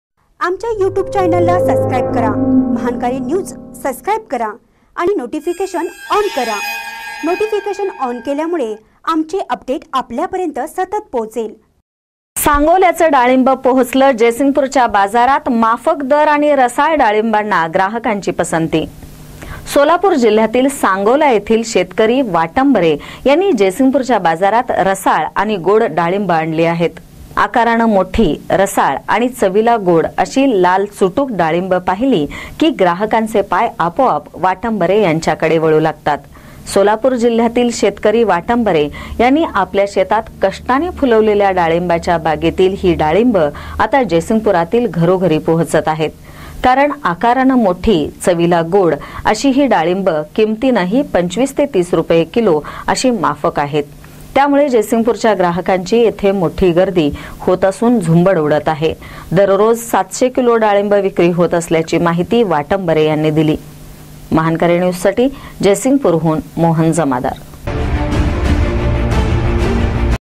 सांगोल्याचं डाळींब पोहचलं जयसिंगपूरच्या बाजारात
सांगोल्याचं डाळींब पोहचलं जयसिंगपूरच्या बाजारात : माफक दर आणि रसाळ डाळींबाना ग्राहकांची पसंती